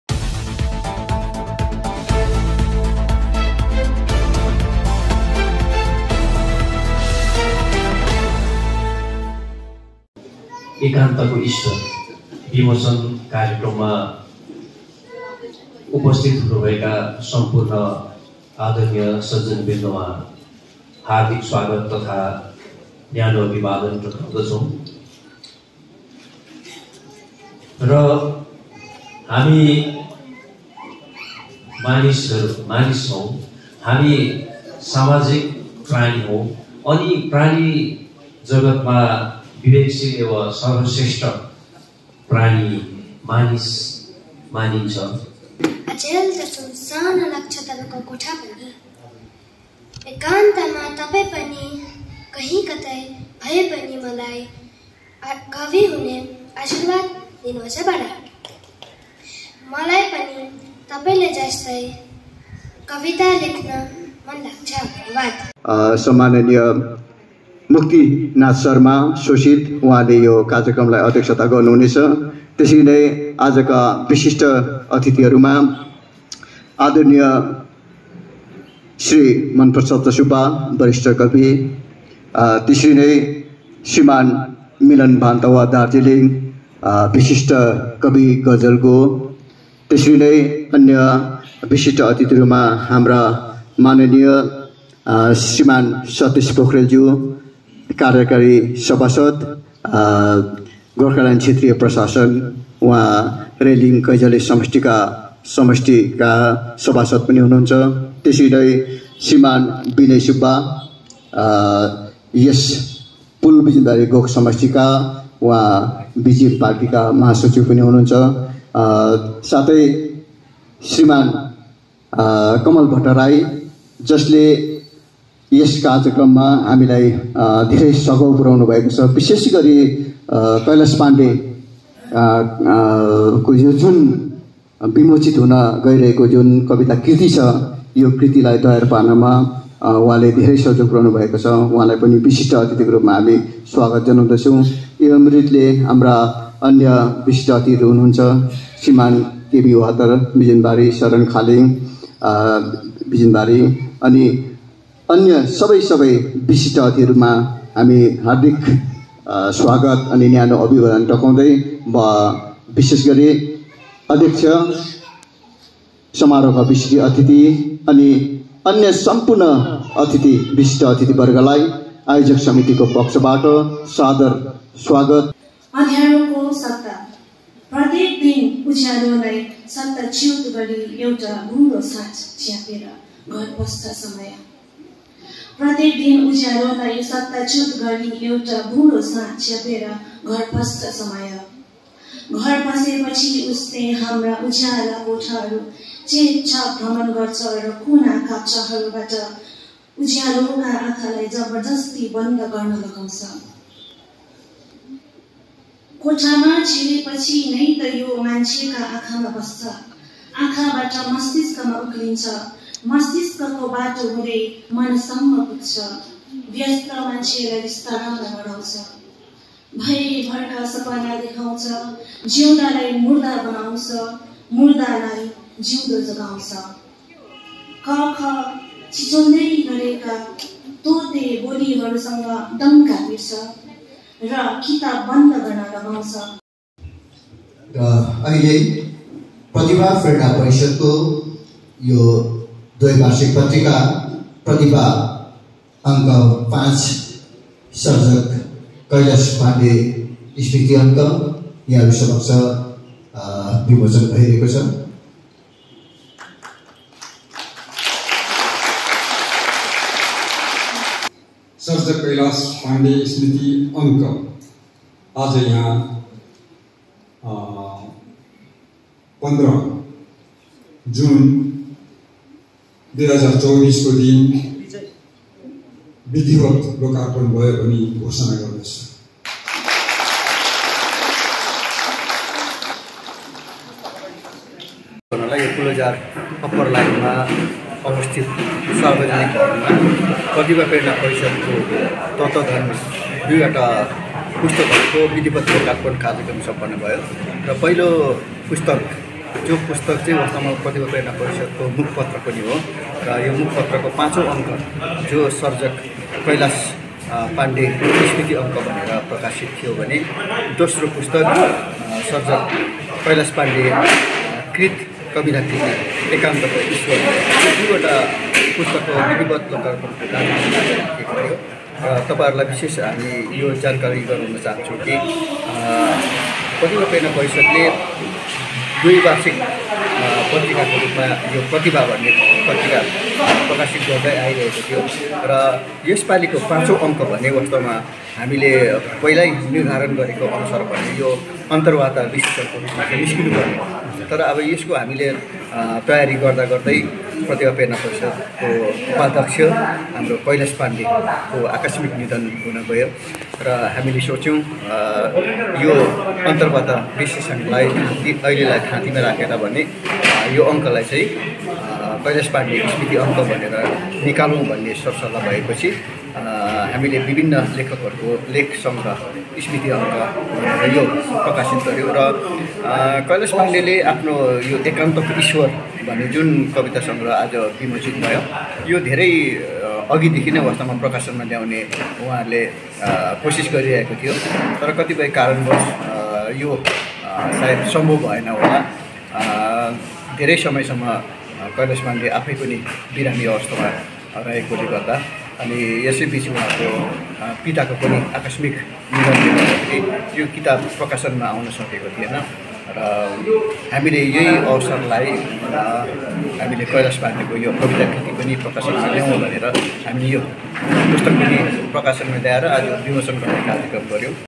कवि कैलाश पाण्डेकृत एकान्तको ईश्वर विमोचन !!!
पुलबजार 15 जून (दीपक राई) एकान्तको ईश्वर कवि कैलाश पाण्डे द्वारा रचित कविता संग्रह अनि कवि कैलाश पाण्डे स्मृति अंक आज विधिवत् रूपमा विमोचन गरियो। मुक्तिनाथ शर्मा शोषितको अध्यक्षतामा सम्पन्न कार्यक्रममा विशिष्ट अतिथीहरु कवि मन प्रसाद सुब्बा, कवि सरन खालिङ, कवि मिलन बान्तवा, सभासद सतिश पोखरेल, सभासद भुपेन्द्र छेत्री स्वo कवि कैलाश पाण्डेका परिवार लगायत विभिन्न स्कूल संघ संस्थाका शिक्षक, सदस्यहरूको व्यापक उपस्थिति थिए।

प्रतिभा प्रेरणा परिषदका सह सचिव जय कुमार राईद्वारा सञ्चालित कार्यक्रममा यसदिन कवि कैलाश पाण्डे द्वारा रचित कविताहरु सुरज शर्मा, आशा लिङ्देन,योगेश ग्याबा, दिवाकर कार्की आदिले वाचन गरे। यसरी नै वन्दना नृत्य विद्यादीप स्कूल पर्बुङका विद्यार्थीहरू प्रकृति सुब्बा,जिया देवान,राखी राई आदिले प्रस्तुत गरे। यसरी नै सांस्कृतिक नृत्य बिजन बारी डिग्री कालेजका विद्यार्थीहरू आशा सुब्बा, इश्मो राई, अनुशा राई, मीरा शर्मा र सञ्जिना राई आदिले प्रस्तुत गरे। यसदिन प्रतिभा प्रेरणा परिषदका सचिव सुजन हाङले कवि कैलाश पाण्डे सँधै आफ्नो कृति कविता, गीत, गजलद्वारा जिउँदो रहिरहने उनले बताए। कवि कैलाश पाण्डे प्रतिभा प्रेरणा परिषदका पनि एक दहरिलो सक्रिय सदस्य रहेको अवगत गराउँदै उनको निधन परिषदको निम्ति अत्यन्त ठुलो क्षति रहेको उनले जनाए। कवि कैलाश पाण्डे एक सर्जक मात्र नभएर गाउँ घर समाजका निम्ति पनि एक दहरिलो खम्बा रहेको जनाउँदै गाउँ घर समाजले एकजना मुख्य अभिभावक गुमाएको आज जनाए। कवि कैलाश अब सशरीर हामी माझ नरहे पनि उनको कृति र सृजनाहरुद्वारा बाँची रहने उनले बताए। यसदिन प्रतिभा प्रेरणा परिषदको पक्षबाट हार्दिक आभार प्रकट गर्दै सम्पूर्ण सर्जक, सहयोगीहरुलाई स्मृति चिन्ह अनि दुई-दुई वटा पुस्तकहरू प्रदान गरियो।